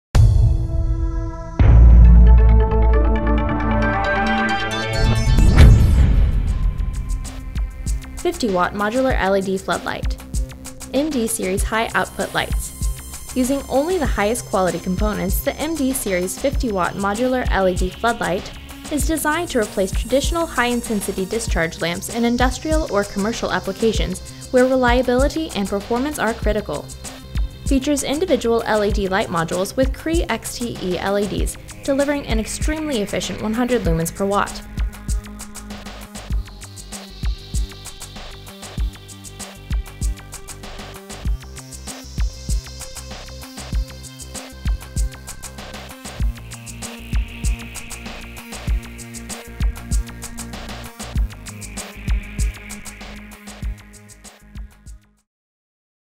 Modular LED Floodlight 50 WATT
Using only the highest quality components, the MD series 50W Modular LED Floodlight is designed to replace traditional high intensity discharge lamps in industrial or commercial applications where reliability and performance are critical. Features individual LED light modules with CREE XT-E LEDs delivering an extremely efficient 100 lumens per watt. Unique, hinged housing that provides easy access to all inner components while the fixture is installed.